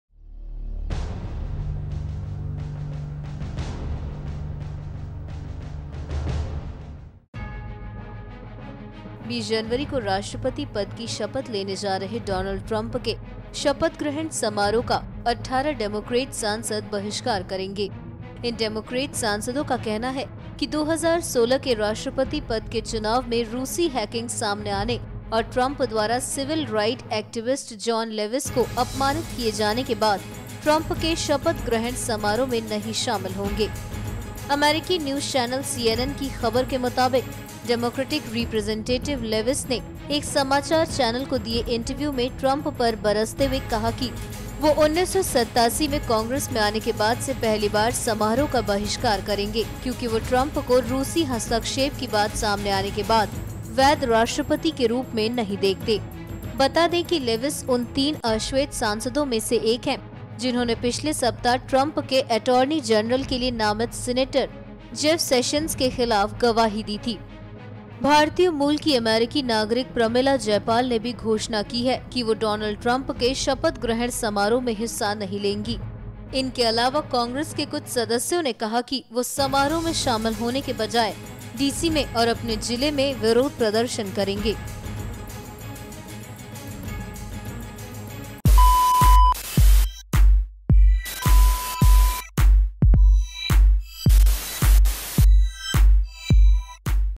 Pramila Jayapal समेत 18 Democratic MP Trump के Oath Ceremony का करेंगे Boycott
At least 18 Democratic lawmakers have said they will boycott US President-elect Donald Trump's inauguration this week, after disclosure of Russia's alleged meddling in the 2016 election and his rebuke of civil rights icon John Lewis, according to a media report Sunday.